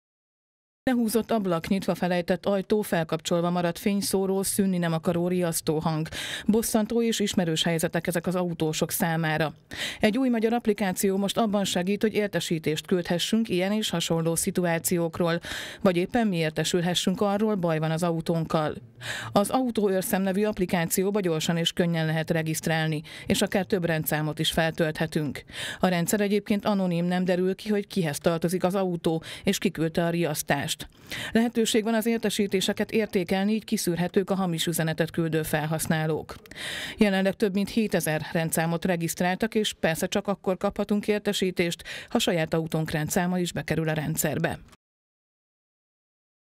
Újabb segítség az autósoknak
Újabb segítséget nyújt az autósoknak egy új magyar applikáció. Rendszám alapján üzenhetünk másoknak vagy kaphatunk értesítést arról, ha valami gond van a parkoló autóval. Természetes az alkalmazás akkor lesz igazán hasznos, ha minél többen regisztrálnak.